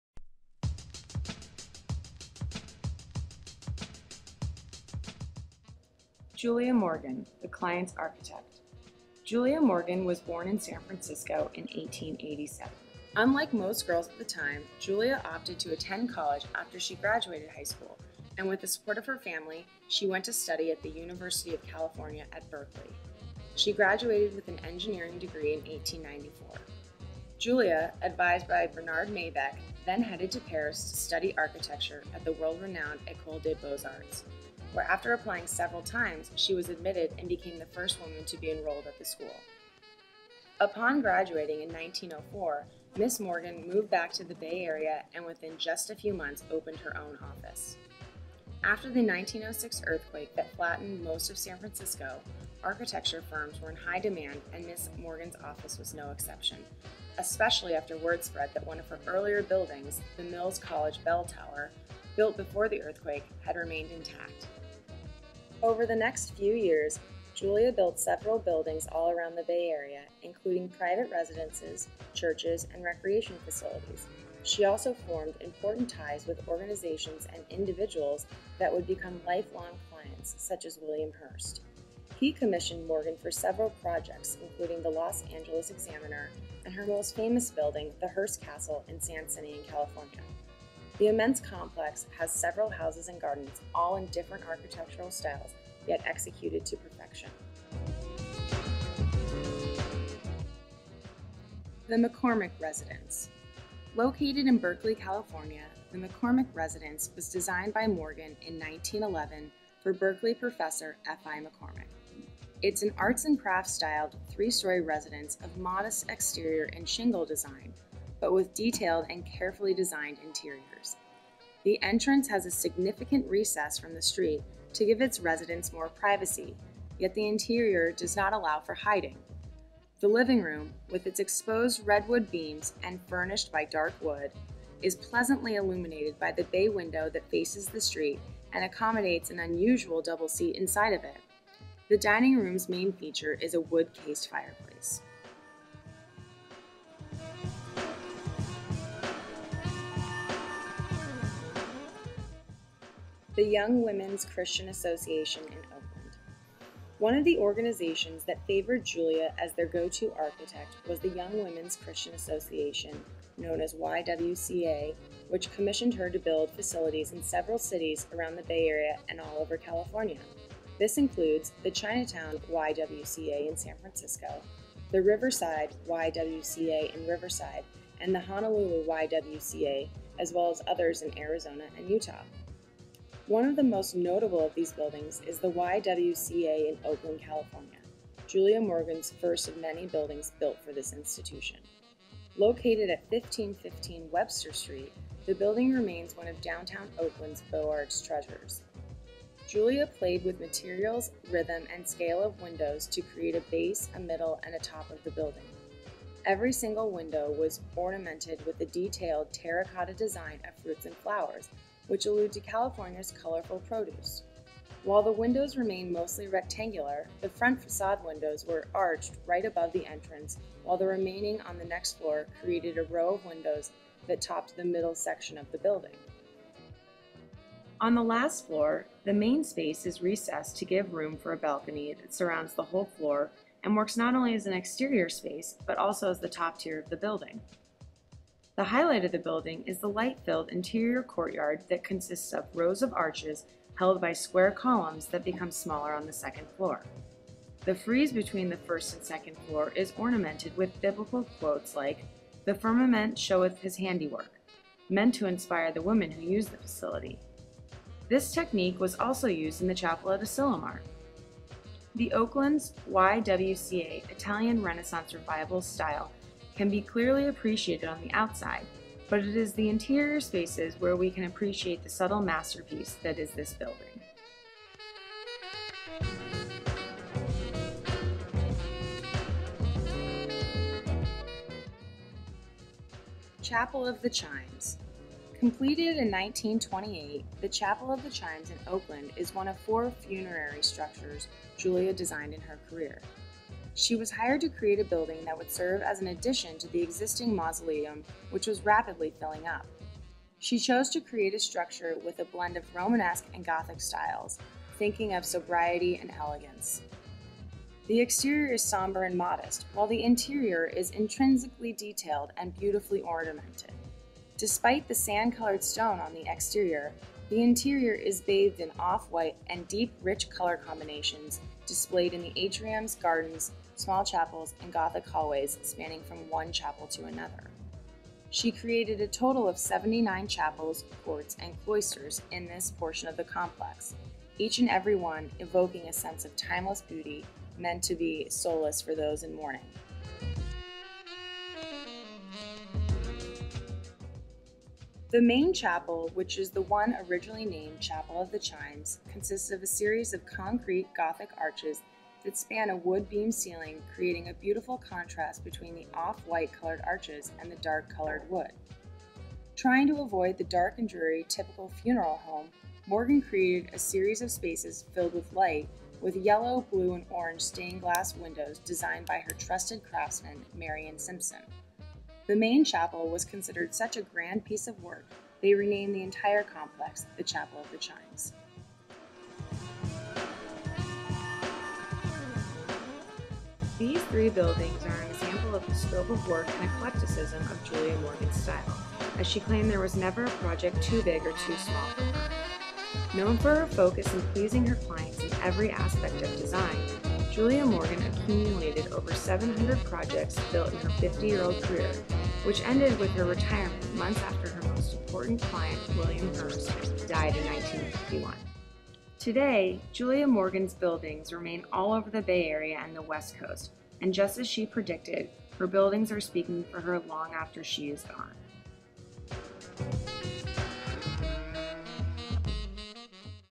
Julia Morgan Bay Area Architect.m4v
Julia Morgan is a turn of the century architect prominent in the Bay Area. Most famous building is the Hearst Castle in San Simeo, CA. This short film is a narrative showcasing Julia's eclectic architectural style and variety of her portfolio of over 700 buildings.